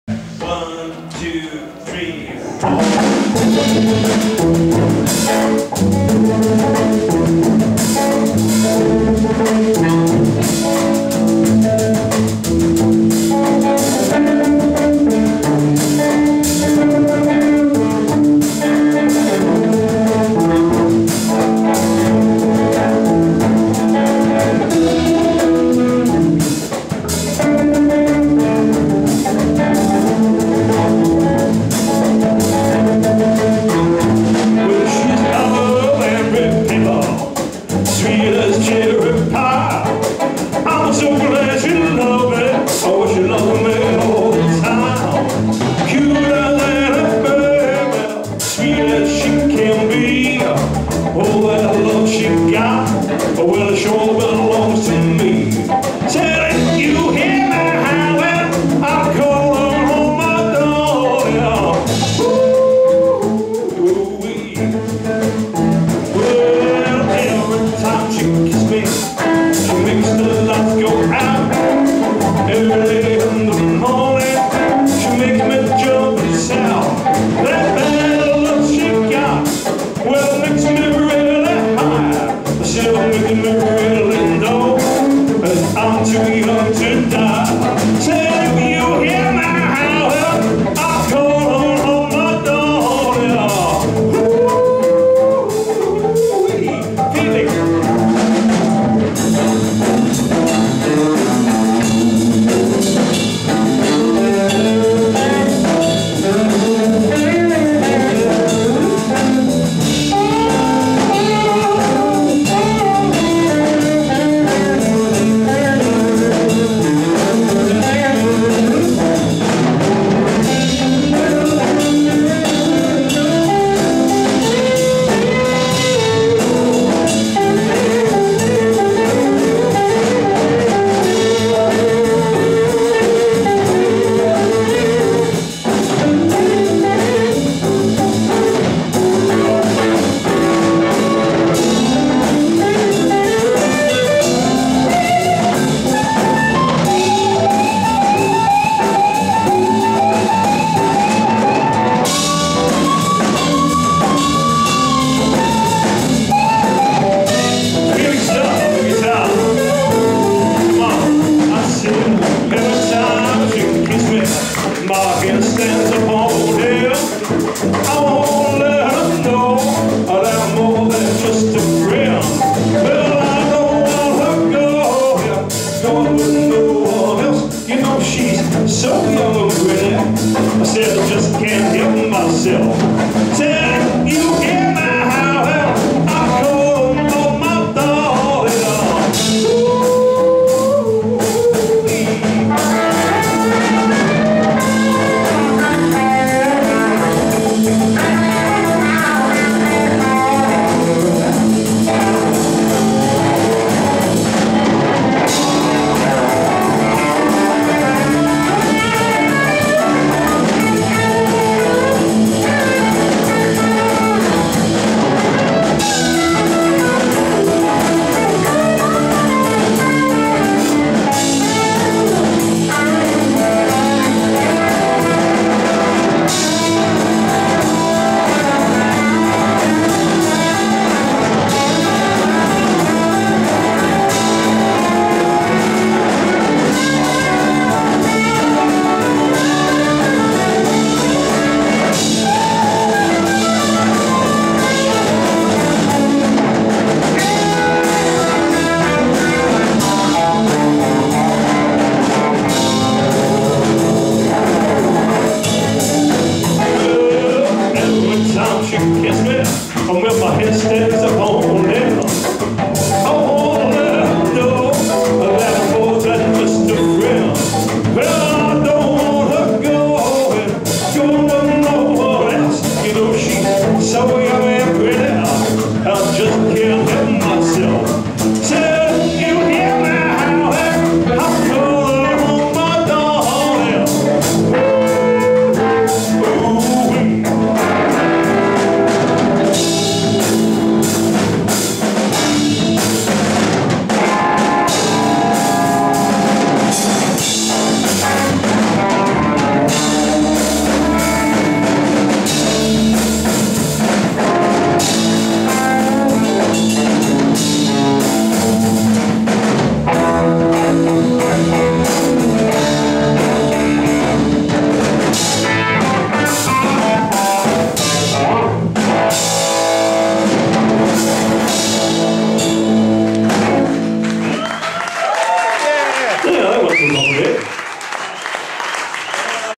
BLUES AT THE COACH AND HORSES 5 3 14
COACHANDHORSESBLUESJAM.COM
STEVIE KING    GUITAR VOC
FELIX KNOTT    GUITAR
KEITH MACK   BASS
HEANYY SUMMERS    DRUMS